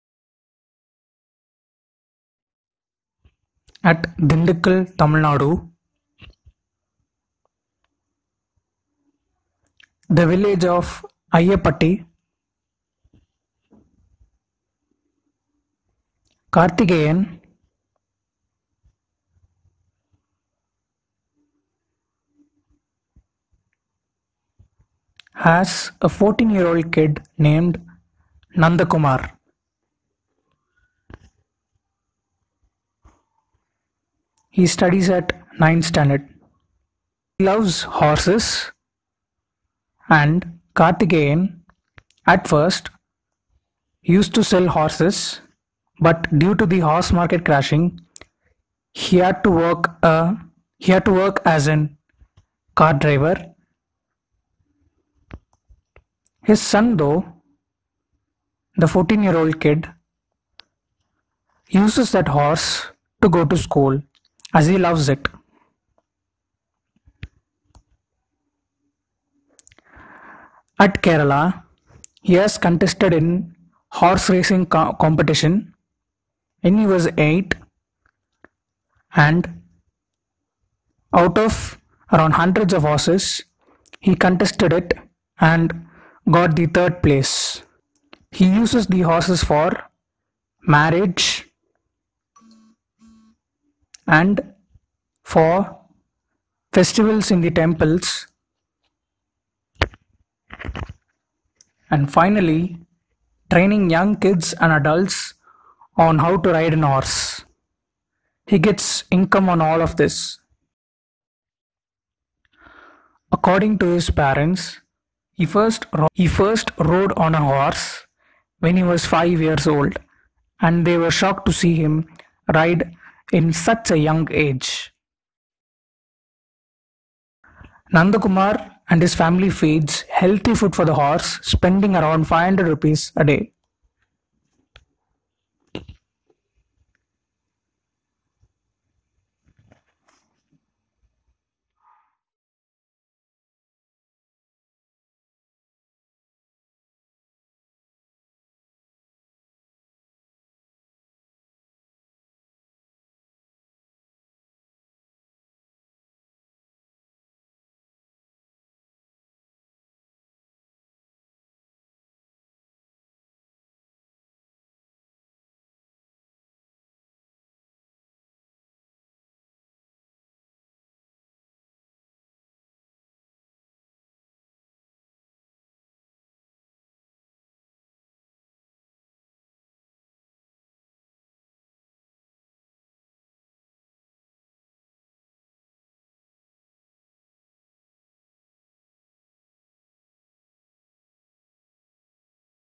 14 year old boy from dindigul rides horse! Uses horse to go to school, teaches people for money!
518th video of our channel.please make sure to like share and hit the bell icon.stay home stay safe.14 year old boy rides horse in dindigul! uses horse to go to school, teaches people for money!